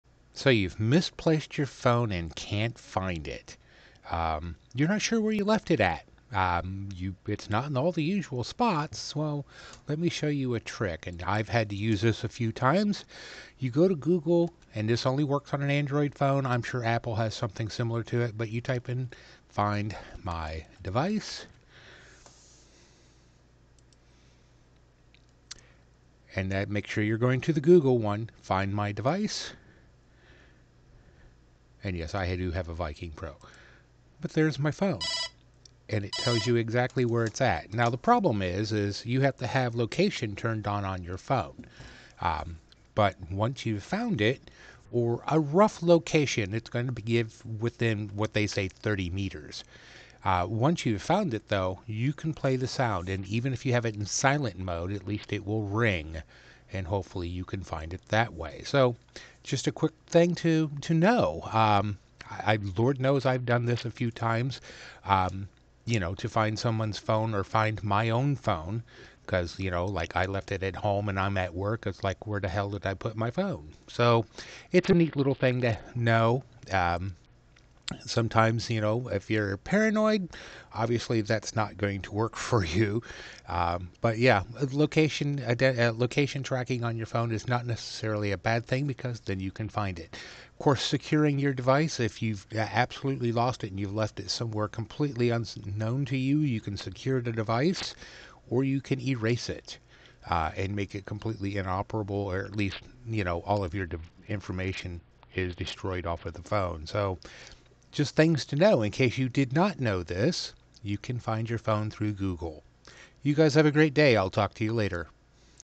09 07 2023 Finding your Andriod Phone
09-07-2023 Finding your Andriod Phone if you have misplaced it